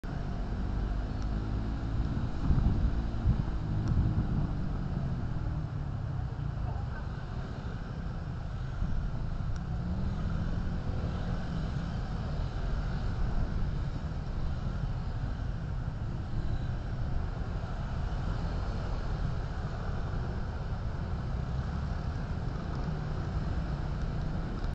過馬路，真難
一共有22台汽機車完全沒停讓，棒